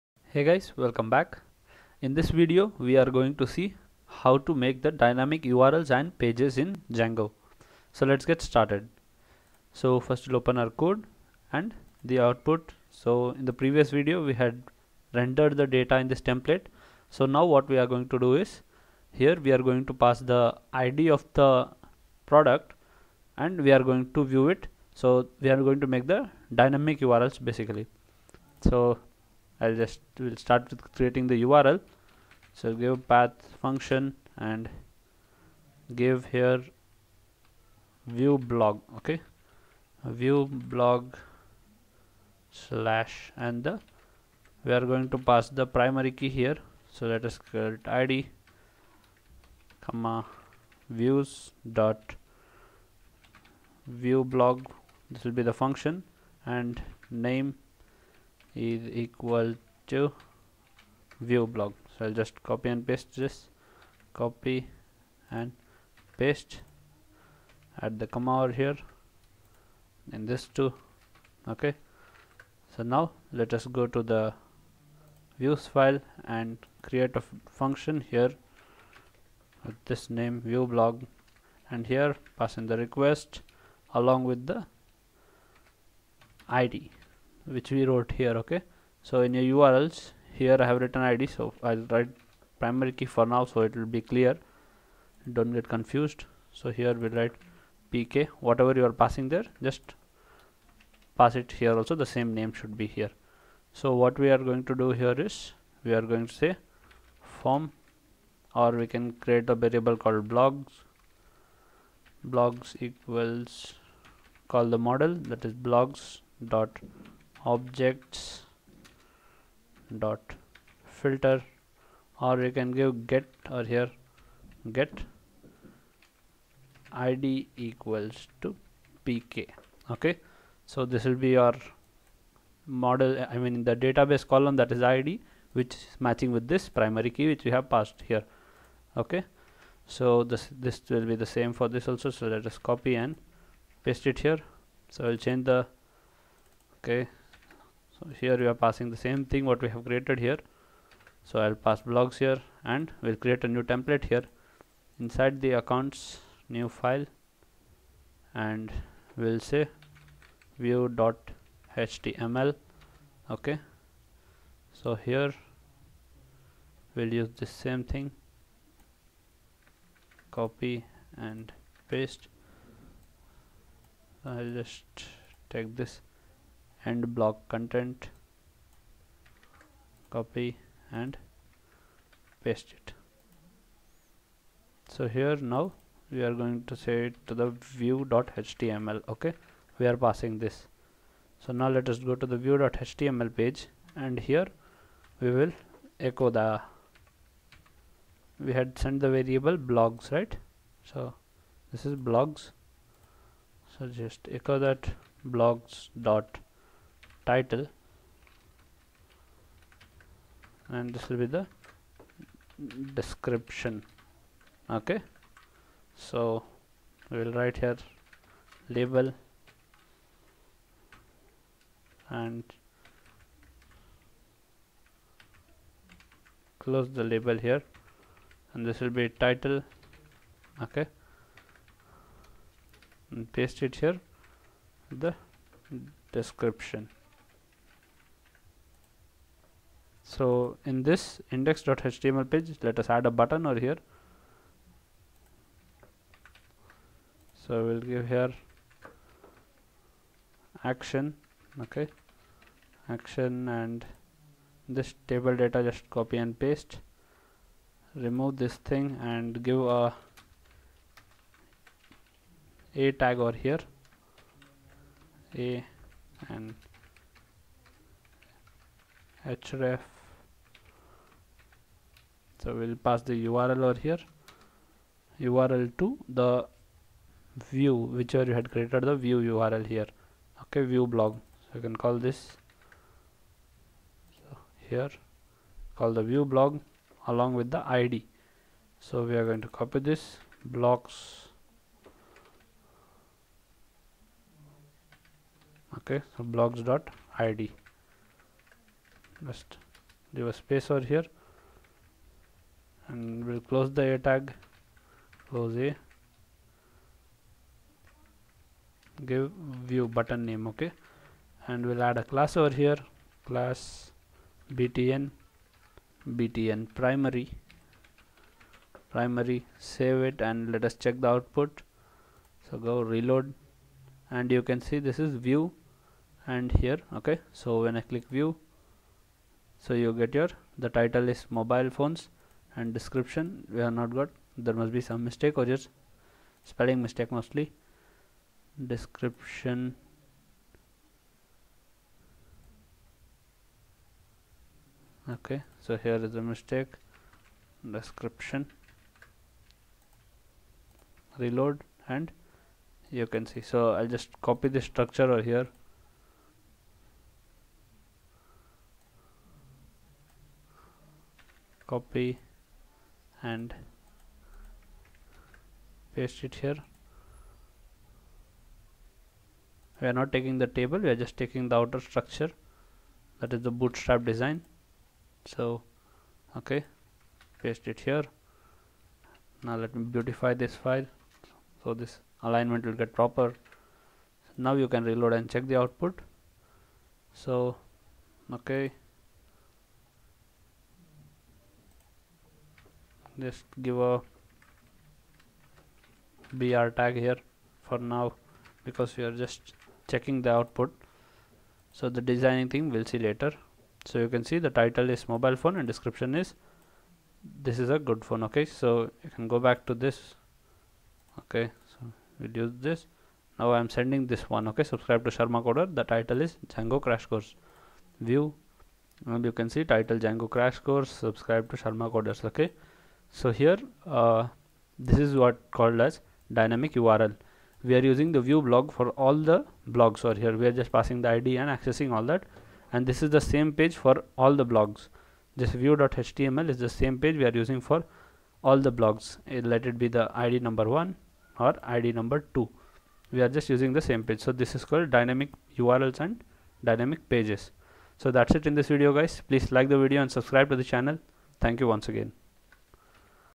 Part 8 : Django Crash Course | Dynamic URLs and Pages | Django framework (3.x) 2021
In this video, you will learn how to create and use dynamic urls and dynamic pages in django. What is dynamic urls and pages also shown with demo.

If you have any doubts or suggestions, drop them in the comments below and i will answer them.